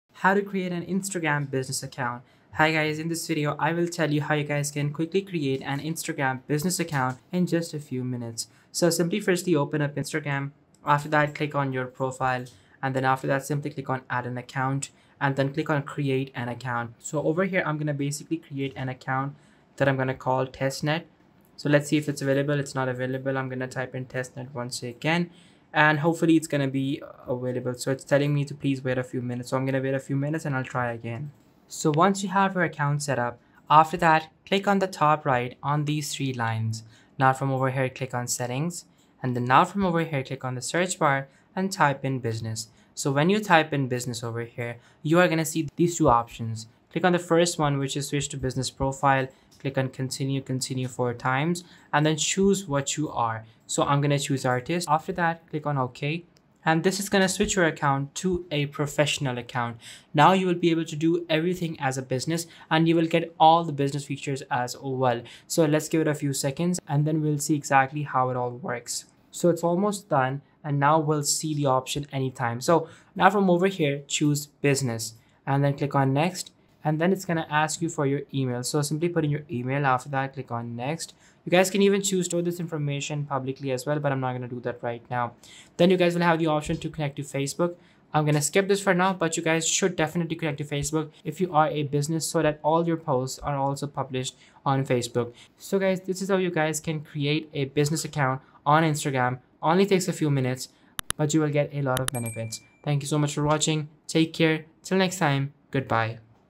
How To Create An Instagram Business Account (2025)
In this video I show you how you can create an Instagram Business Account. This is super easy to do and only takes a few minutes.